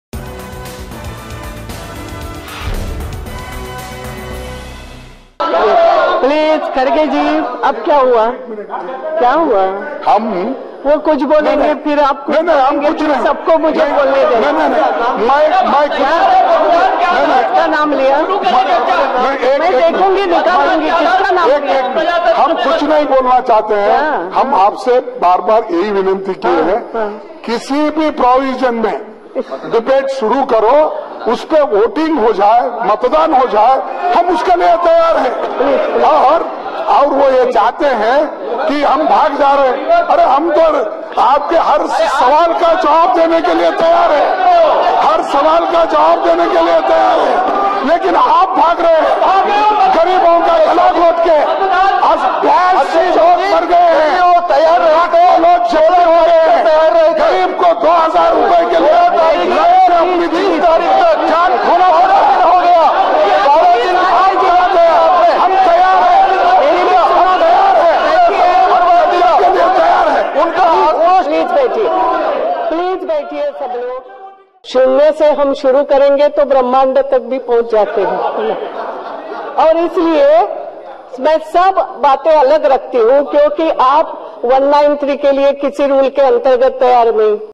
Mallikarjun Kharge vs Ananth Kumar | Lok Sabha Speaker To Start Debate At Any Provision
Mallikarjun Kharge also urged the Speaker to start the debate under any rule which entails voting.  words observed between Congress Leader Mallikarjun Kharge and BJP Leader Ananth Kumar over Demonetisation Debate.